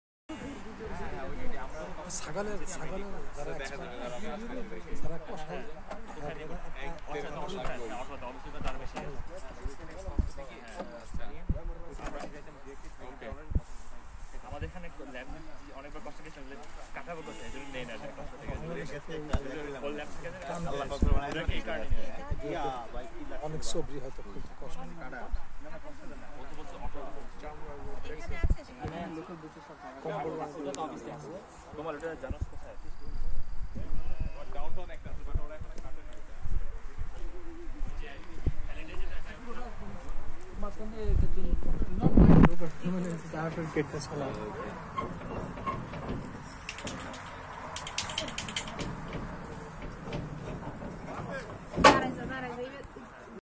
Canada Cattle Slaughtering For Eid777+ ~Rahat Pof | 777+ TV | Newfoundland | Eid ul Adha
Canada Eid ul Adha in St John's, Newfoundland 🇨🇦 Canada Live 777+ Smile Symbol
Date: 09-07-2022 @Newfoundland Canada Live
~Reporter: Rahat Hasan Pof, Canada
~Location : Newfoundland, Canada
~Video Send Media: WhatsApp from Canada
~Video Upload Location: London
~Channel: 777+ TV
Ref: 777+ Smile Symbol @London